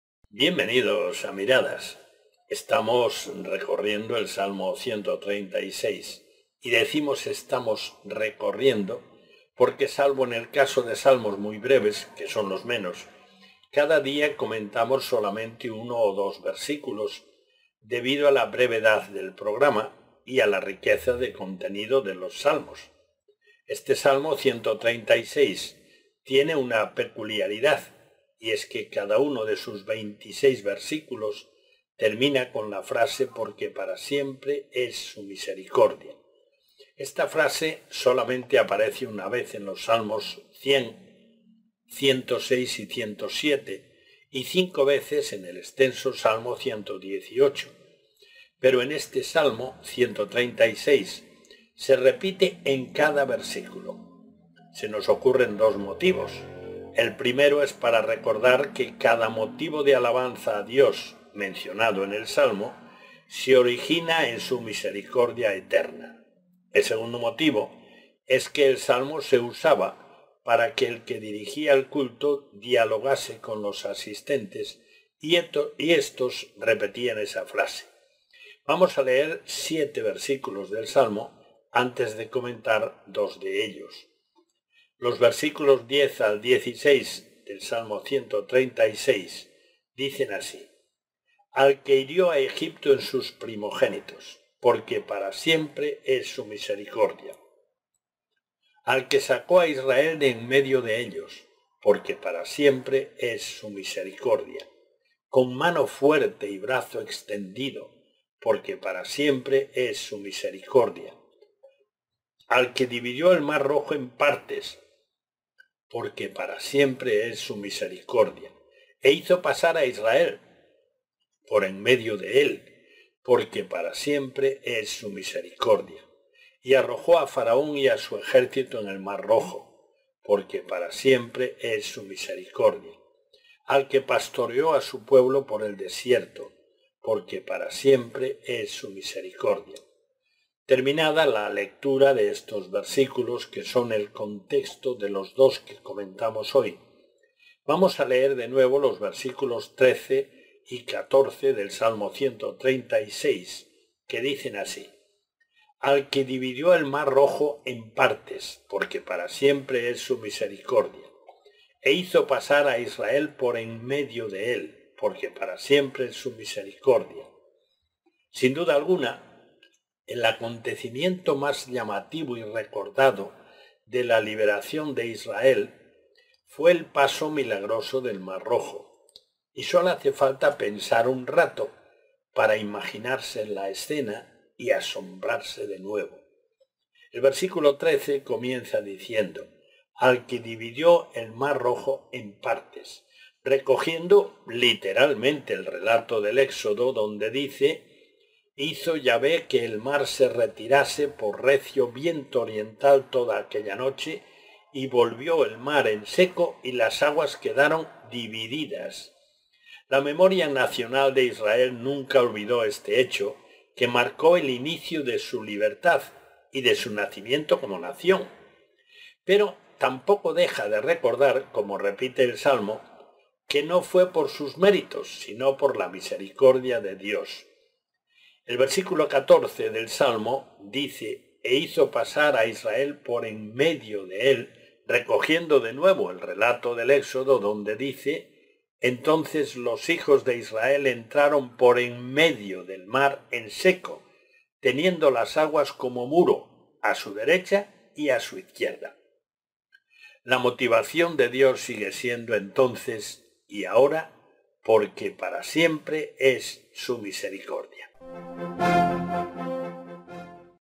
Salmo 136 - Milagro misericordioso (918)
Este salmo ciento treinta y seis tiene una peculiaridad y es que cada uno de sus veintiséis versículos termina con la frase: “Porque para siempre es su misericordia”.

Esta frase solamente aparece una vez en los salmos cien, ciento seis y ciento siete, y cinco veces en el extenso salmo ciento dieciocho, pero en este salmo ciento treinta y seis se repite en cada versículo.

- Aplicación gratuita